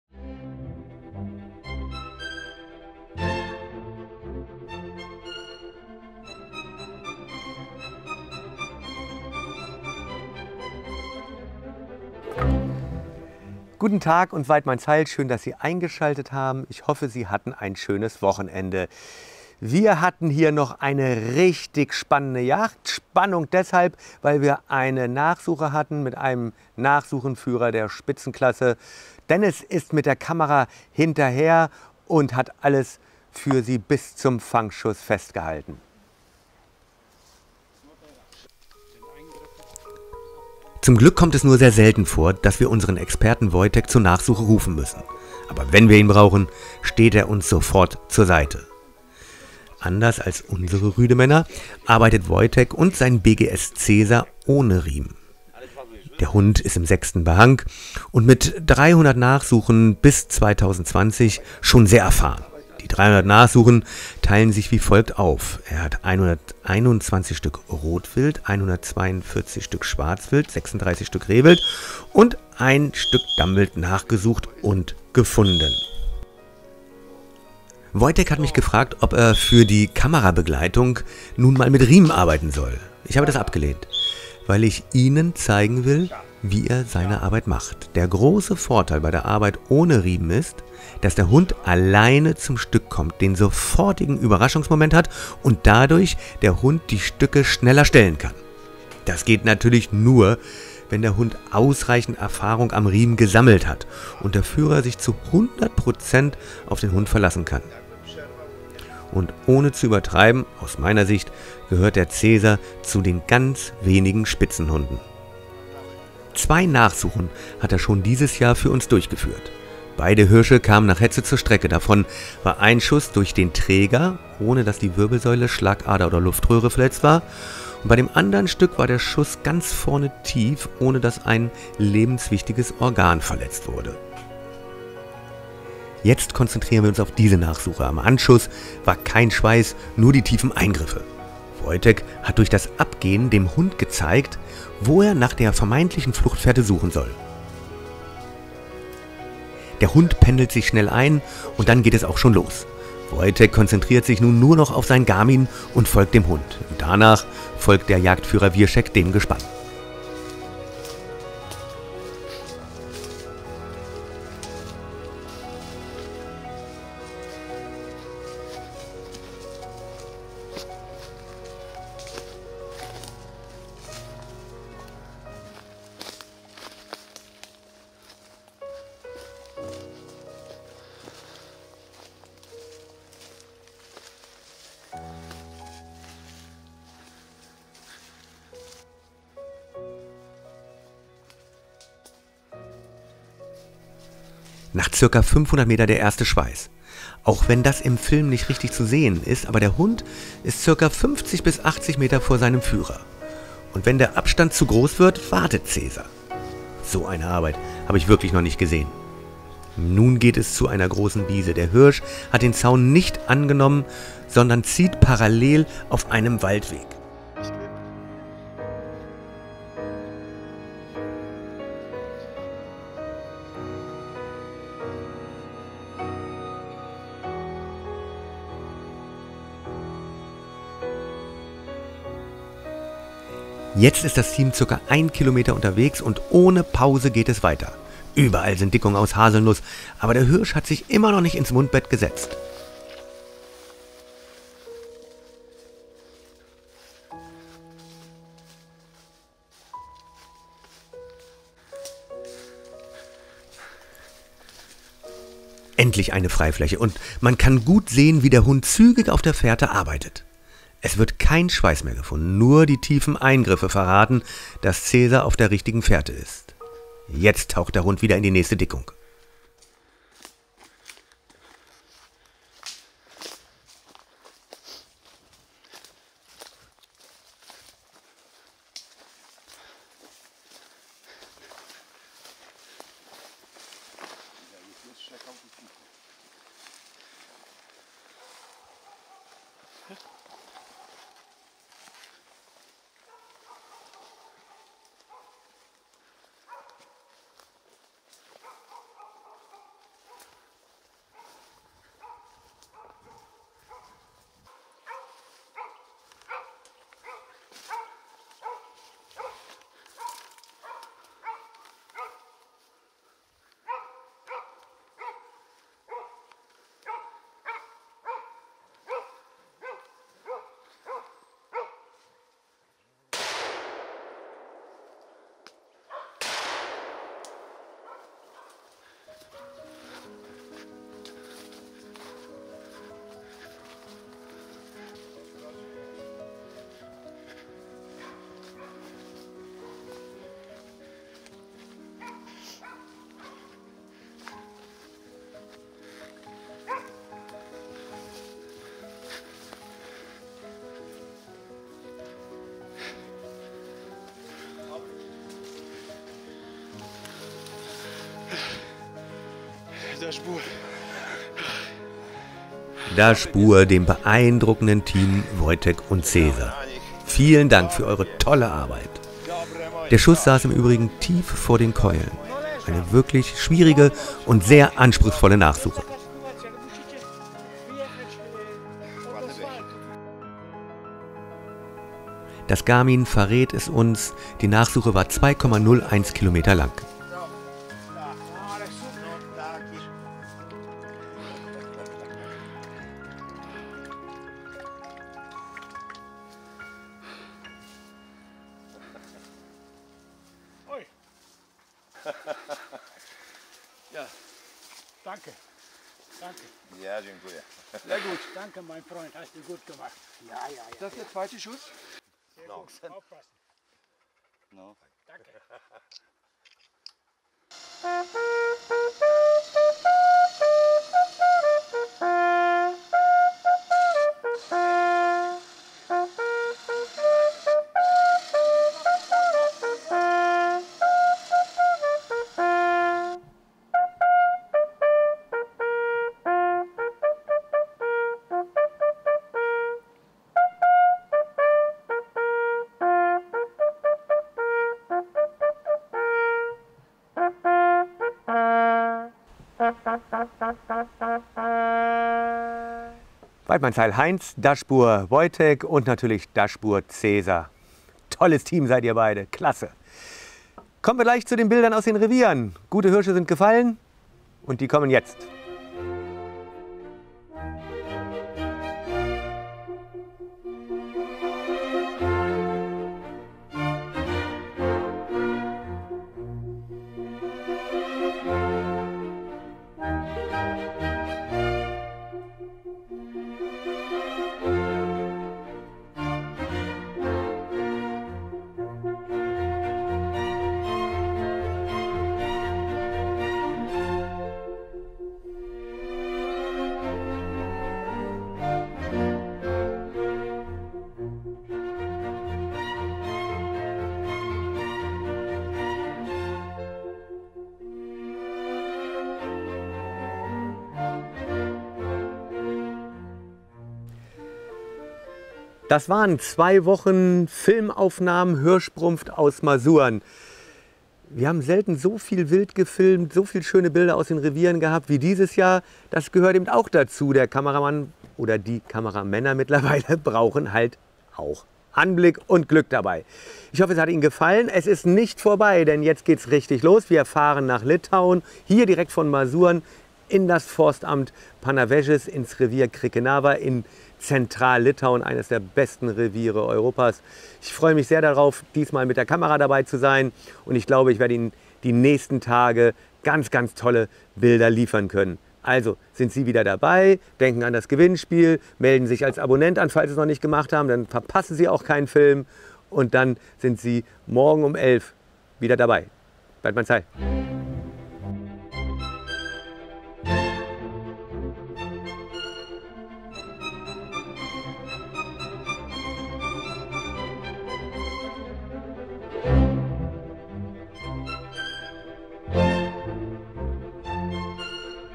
Nachsuche mit Fangschuss und Hetze auf kapitalen Rothirsch
Wir konnten eine Nachsuche auf einen Hirsch mit der Kamera begleiten. Kein Schweiß konnte am Anschuss gefunden werden und somit musste ein professioneller Hundeführer hinzugerufen werden. Erleben Sie die ruhige und hervorragende Arbeit des Gespannes mit.

Vielen Dank für´s Zuschauen & Waidmannsheil

Ihr
Jörg Eberitzsch